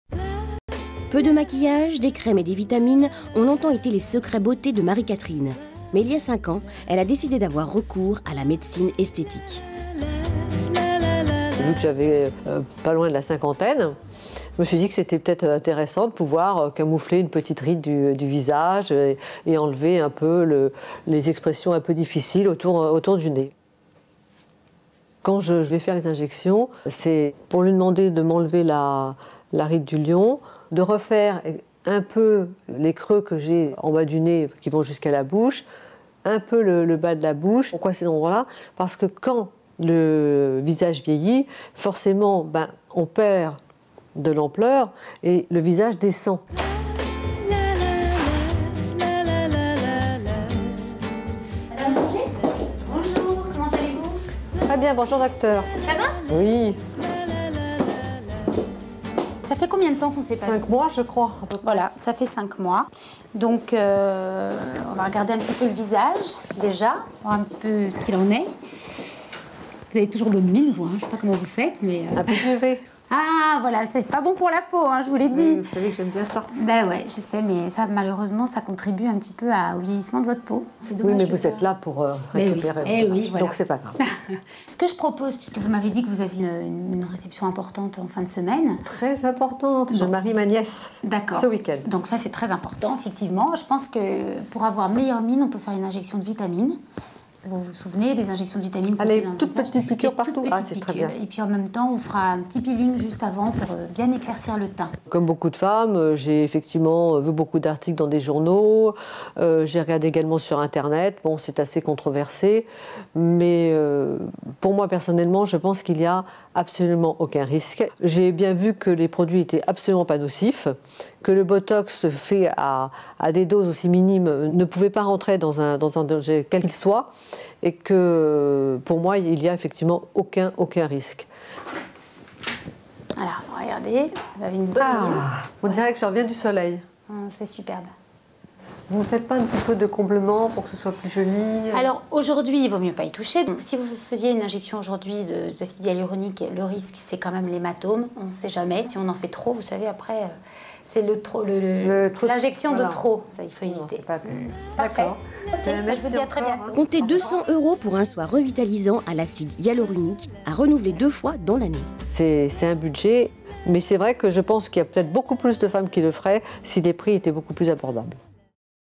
Médecine esthétique : le bistouri c'est pas automatique ! Explications de Vivolta
Effacer ses rides ou ses poches sous les yeux, remodeler l'ovale de son visage ou tout simplement avoir "bonne mine", le tout sans avoir recours au bistouri : c'est ce que propose la médecine esthétique. Injections ou laser représentent plus de 80% de l'activité des chirurgiens et médecins "plasticiens" aujourdhui... Explications sur Vivolta